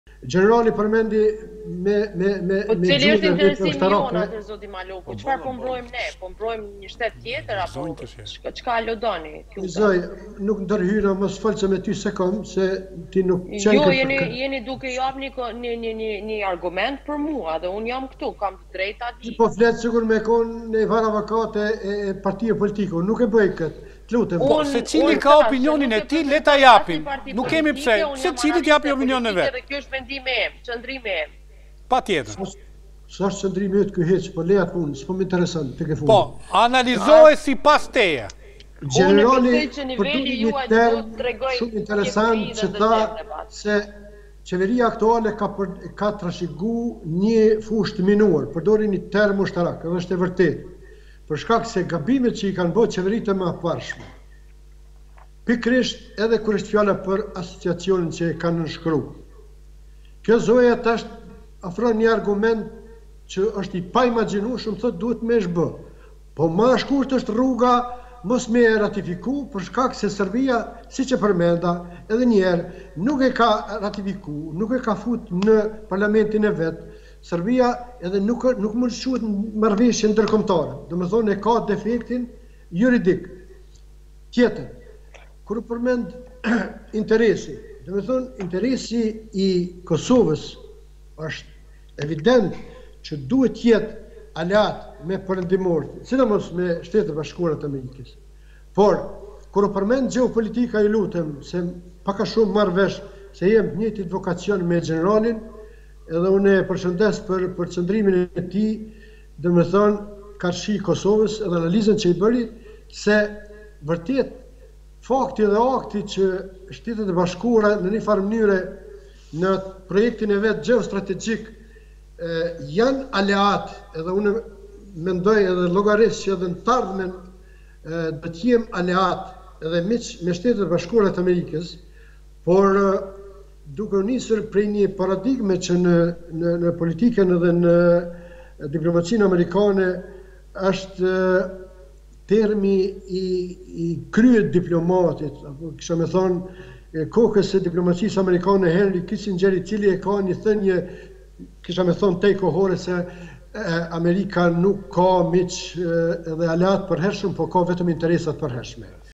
"Amerika nuk ka miq dhe aleatë, por ka vetëm interesa të përhershme", Maloku | Pa Filtra
𝐀𝐮𝐭𝐨𝐫 : NTV